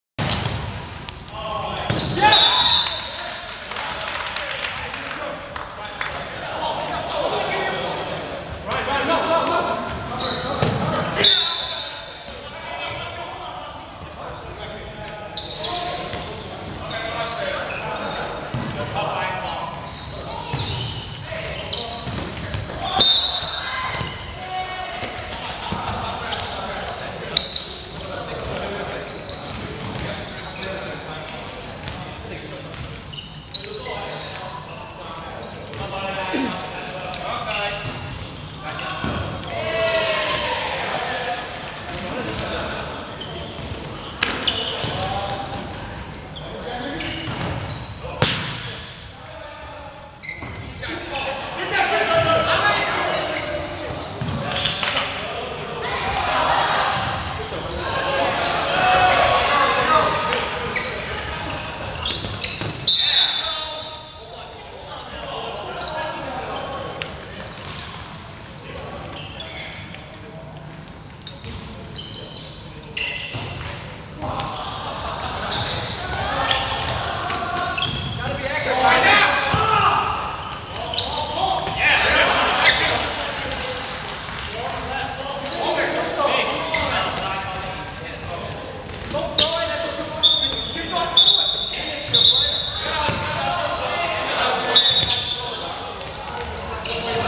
Teabags vs Boiling Guys Game 8 of 16
Teabags win set, score 6-2